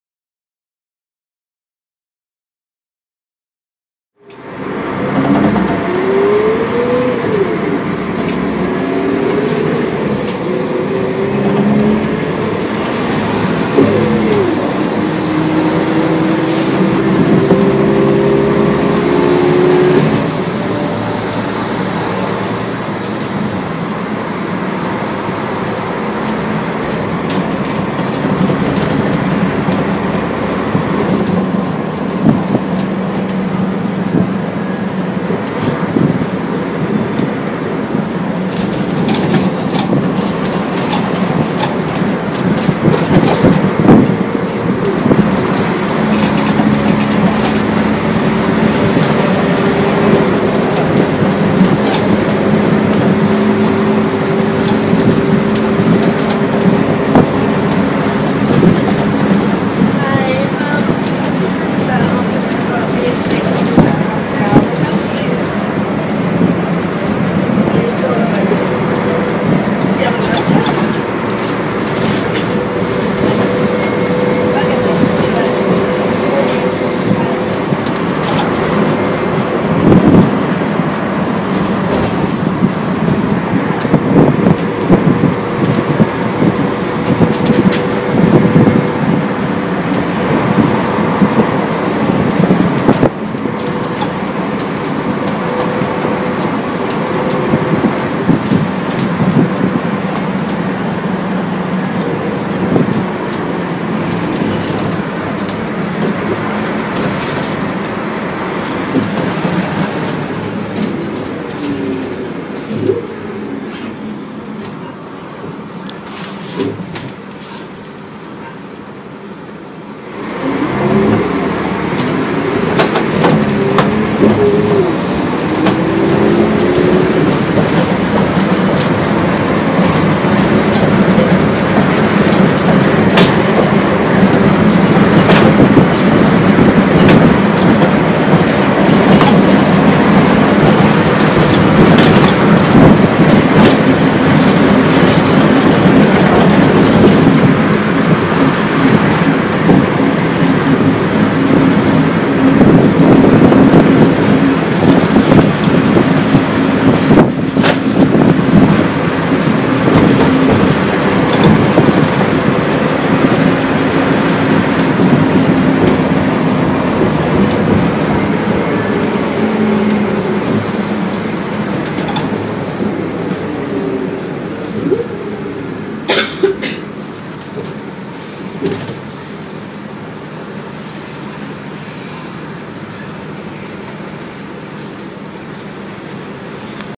Hawkesbury Valley Buses m/o 7488 (Volvo B10MSE - Volgren CR221)
Taken 16/11/13 on Western Line rail bus duties between Parramatta and Blacktown. This one was built in 1999 and is your average B10M.